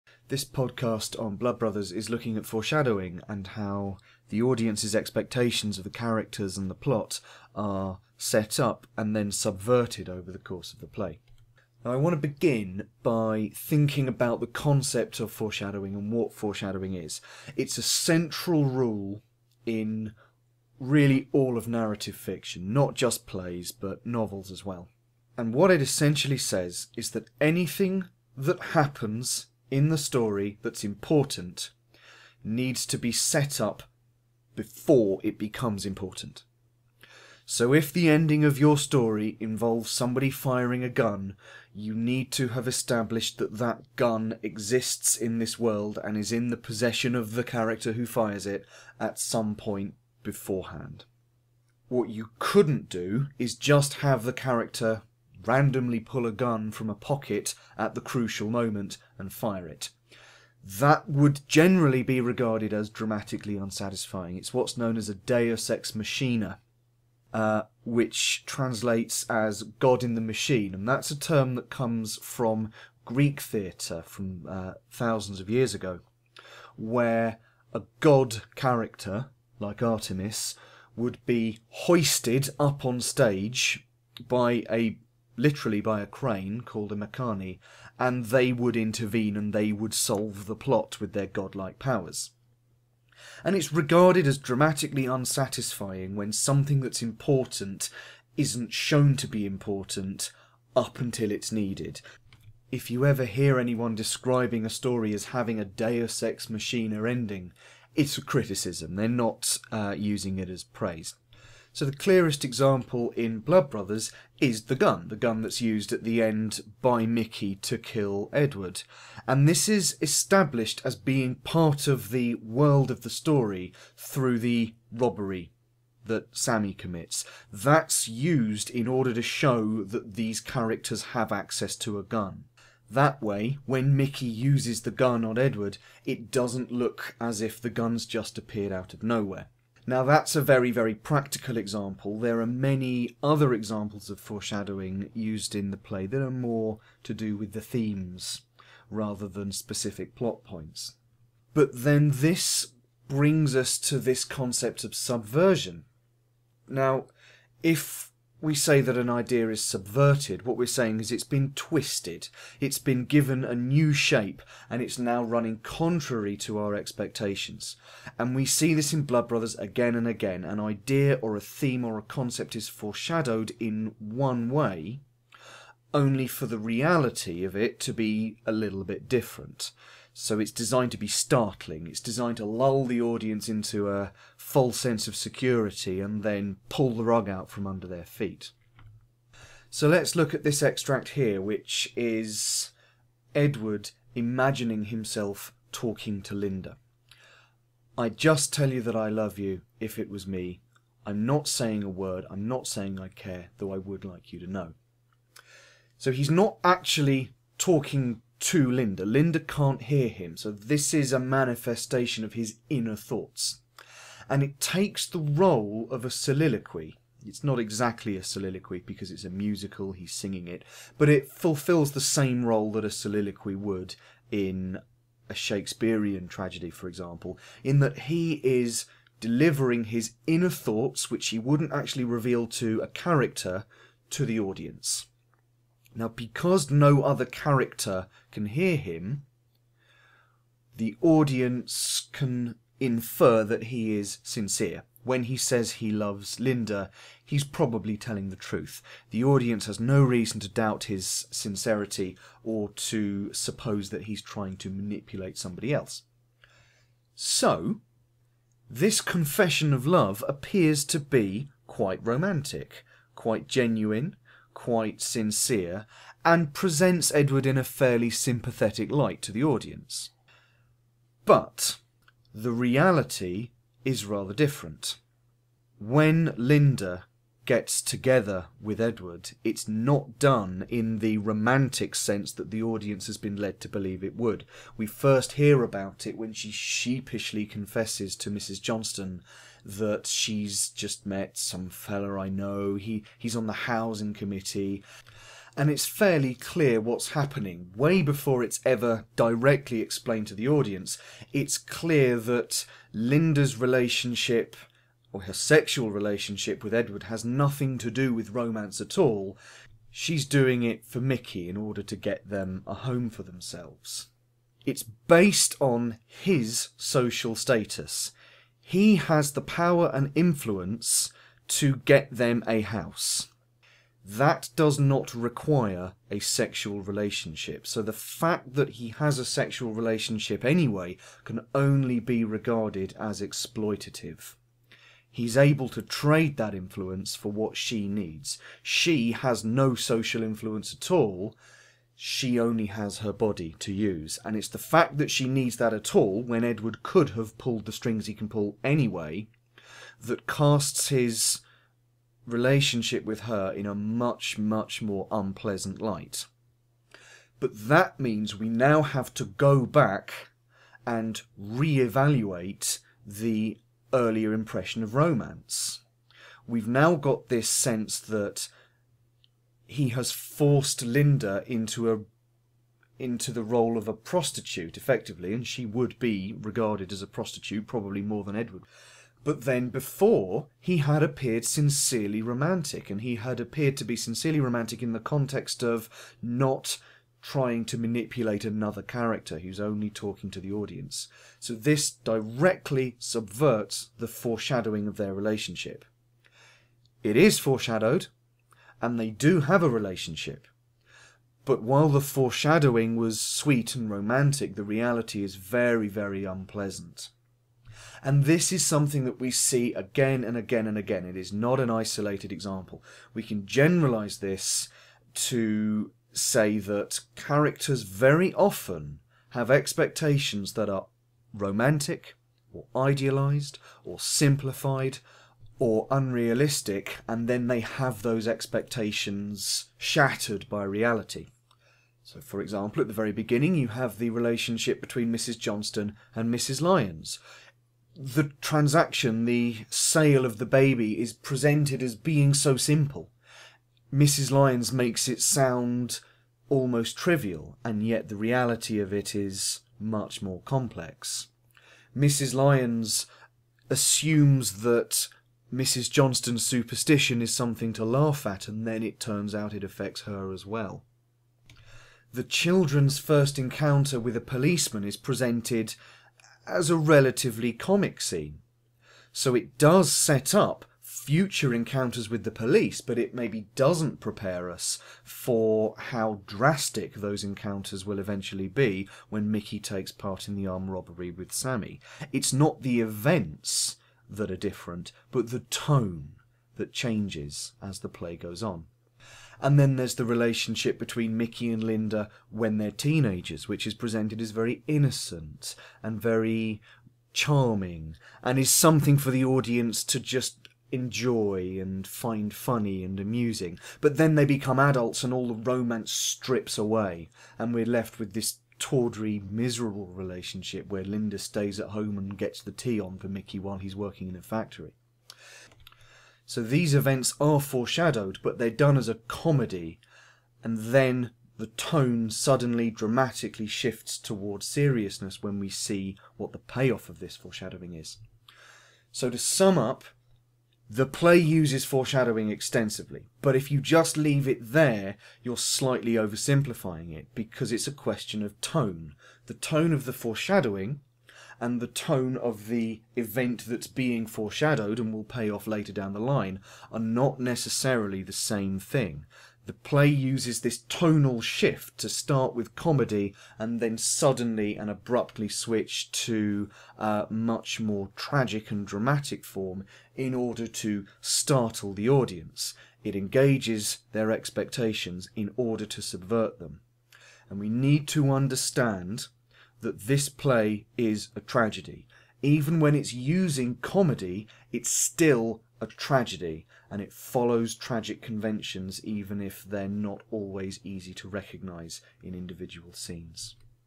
Blood Brothers - Foreshadowing and the Subversion of Expectations
How events are foreshadowed, and how the audience's expectations are subverted in the play.